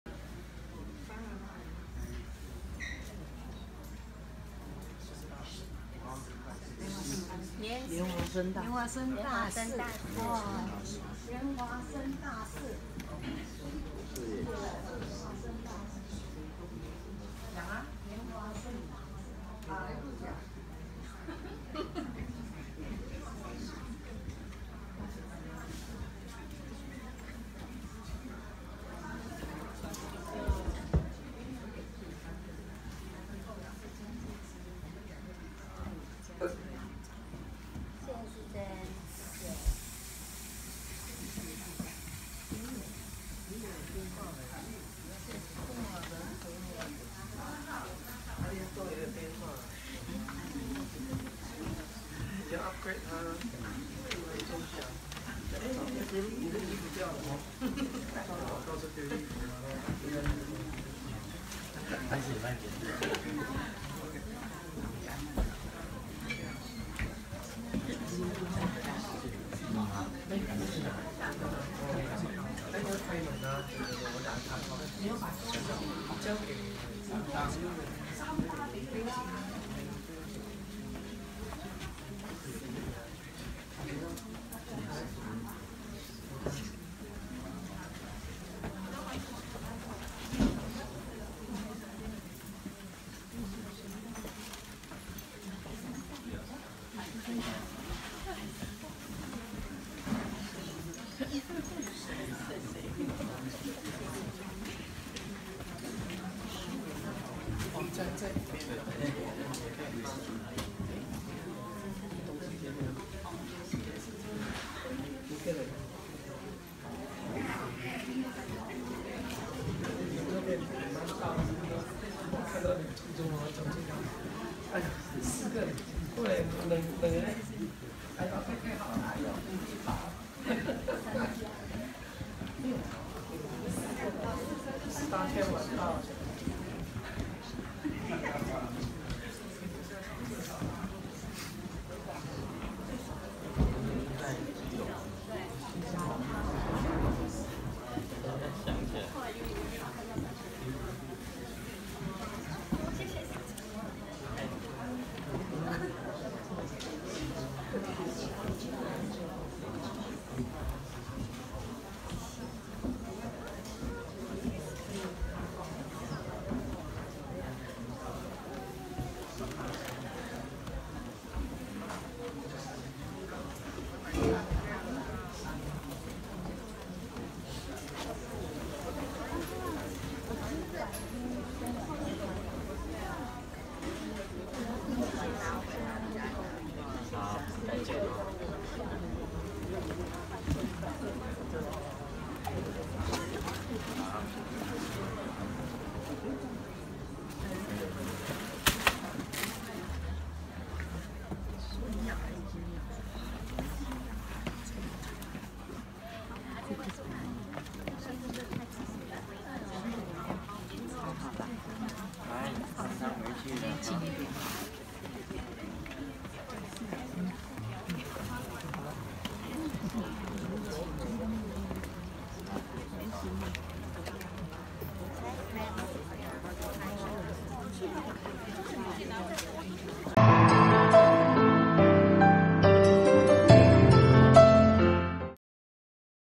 20161114聖尊蓮生活佛盧勝彥晚餐後「蓮華生大士」下降給大眾摩頂加持－TBSN高清版
20161114聖尊蓮生活佛盧勝彥晚餐後「蓮華生大士」下降給大眾摩頂加持－TBSN高清版